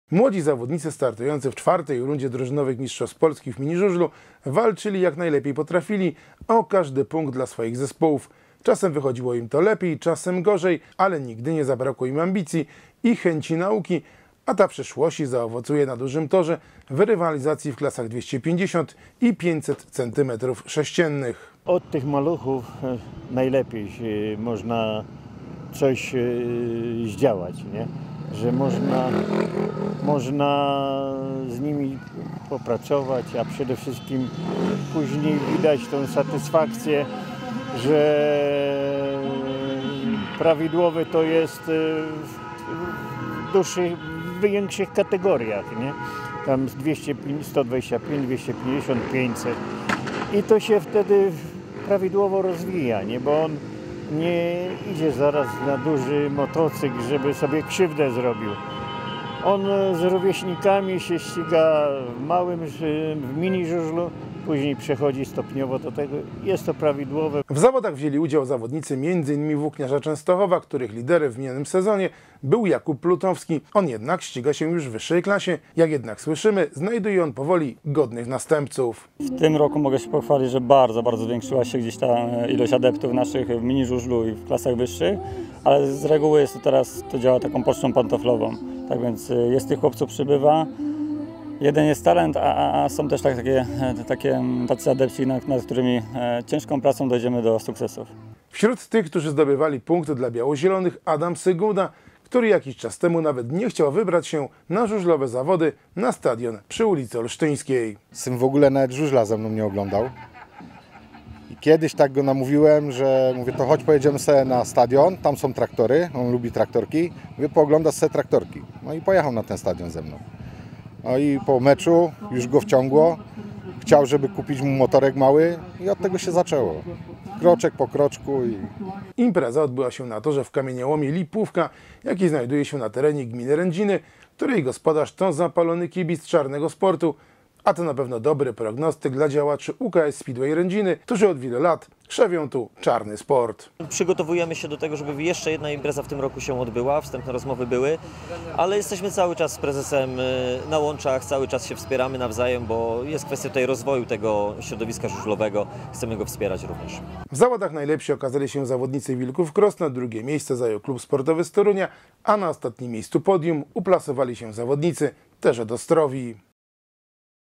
V rundy DMP w mini żużlu w gminie Rędziny
Gmina Rędziny była gospodarzem IV rundy Drużynowych Mistrzostw Polski w mini żużlu.